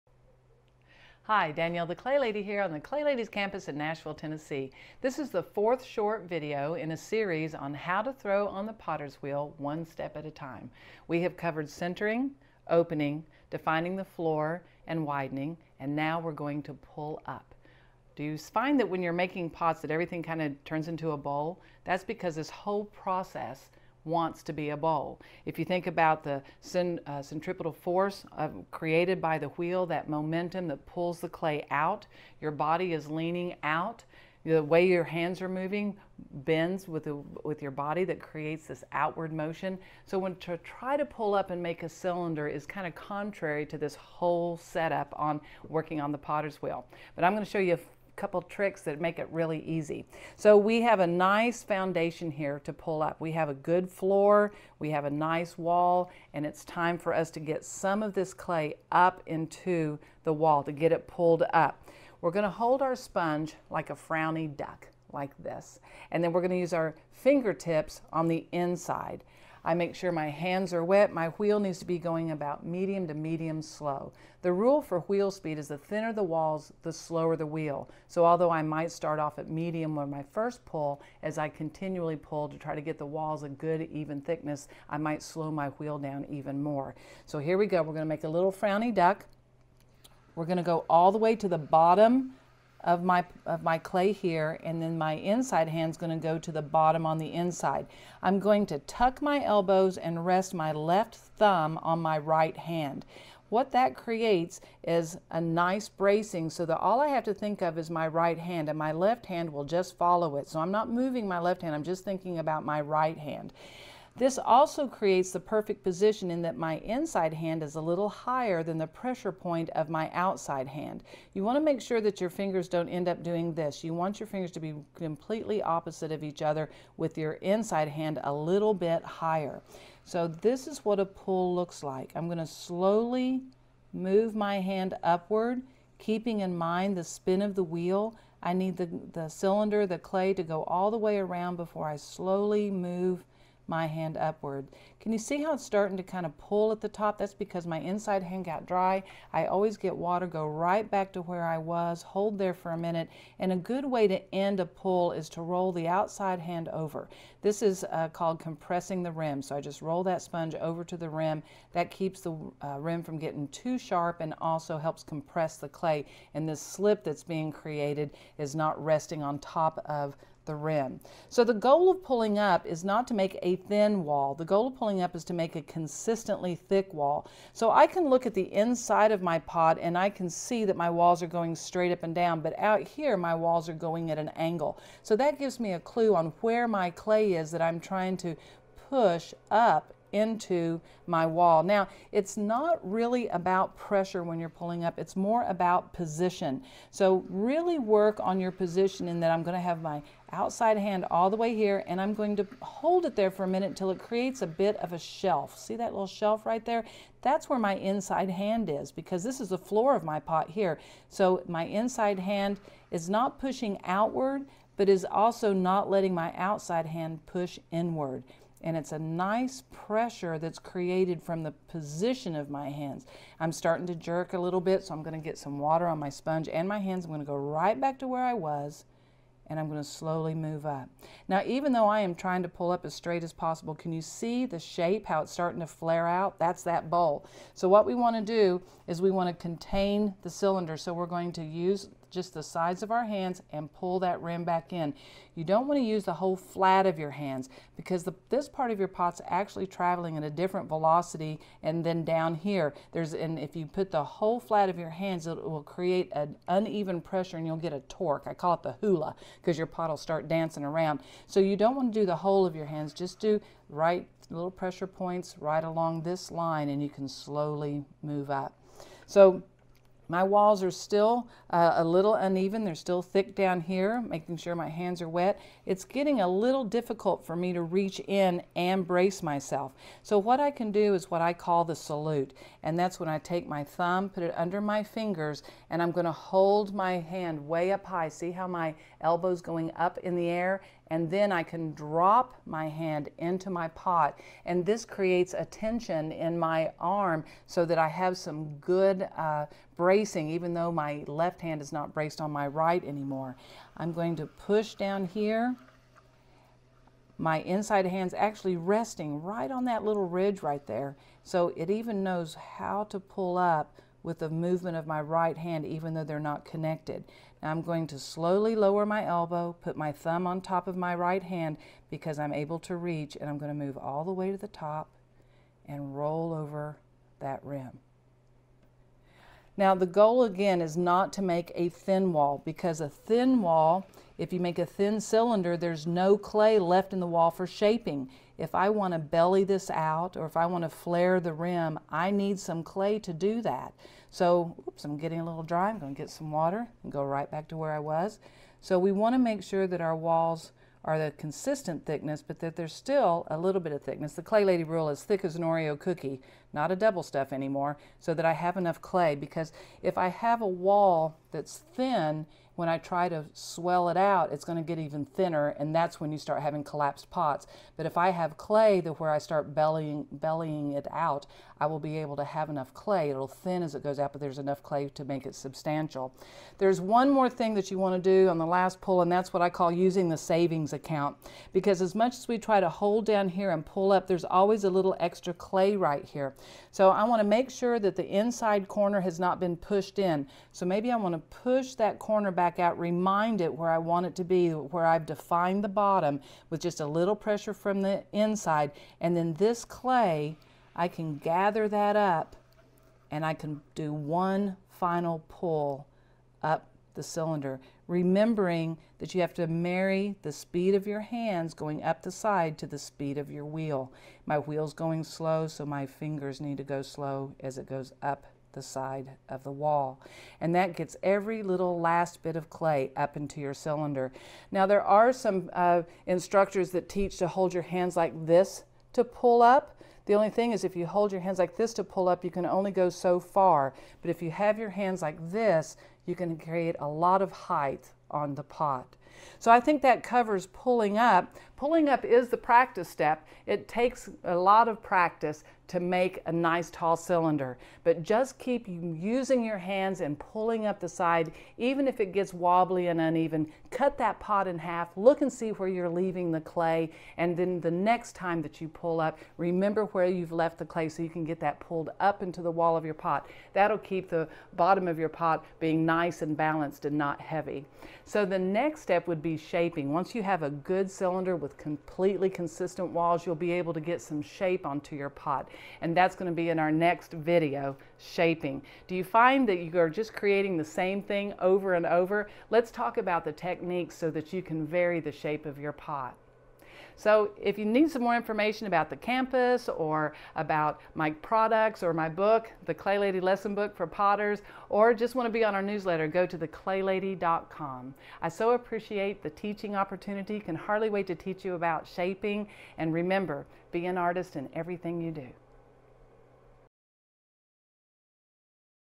Pulling Up: Throwing on the potter's wheel with The Clay Lady, One step at a time
Part 4 of 6 in a series called Throwing on the Wheel, One step at a time with Danielle McDaniel ~ The Clay Lady
In celebration of Danielle's 40th year in clay, The Clay Lady re-releases her beloved videos on the basics of throwing on the potter's wheel. She demonstrates centering, opening, defining the floor, pulling up, shaping, final look, and trimming.

Check out her YouTube Channel for more videos and tips in the studio.
Visit her website to learn more about The Clay Lady and The Clay Lady's Campus in Nashville, Tennessee.